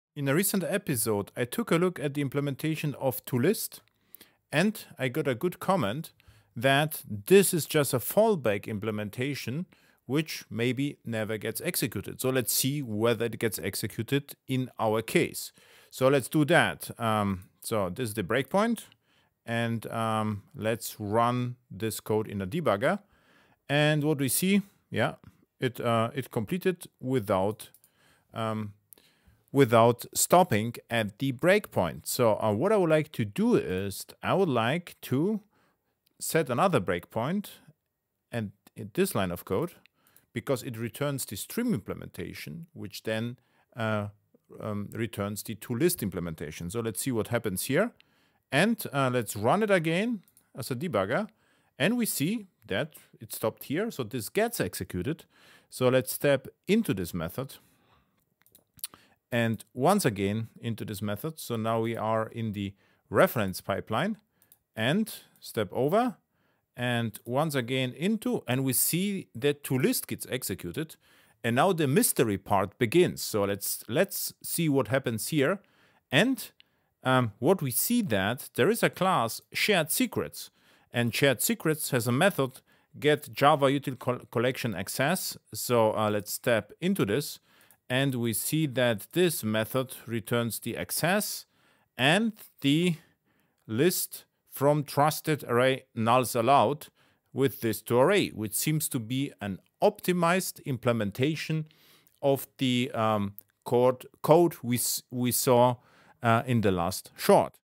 Stream.toList has Secrets
The mysterious implementation of the Stream.toList() method.